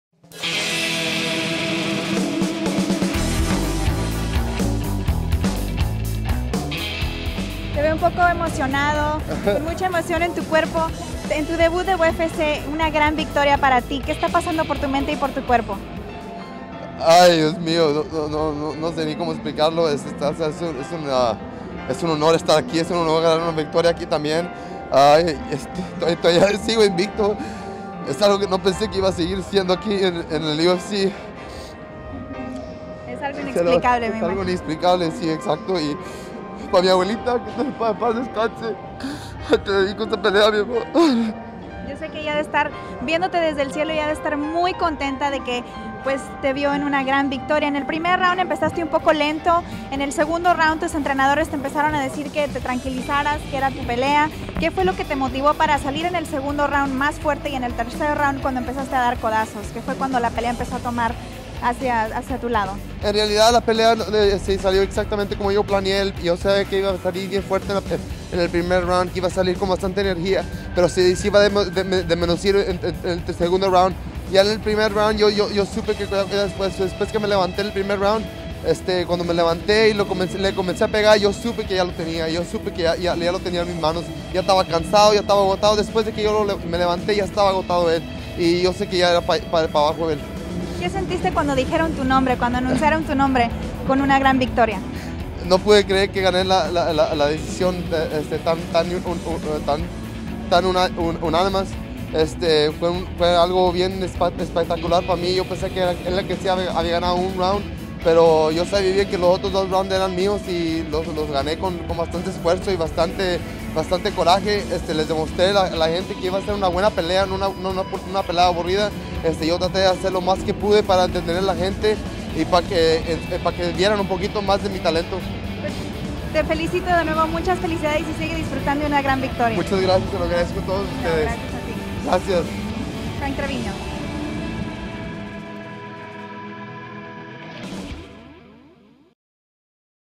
UFC 171: Frank Trevino Entrevista Posterior
El mexicano Frank Treviño debutó con victoria en UFC 171 en Dallas y platicó con las cámaras de UFC Network sobre lo importante que fue para él esta victoria.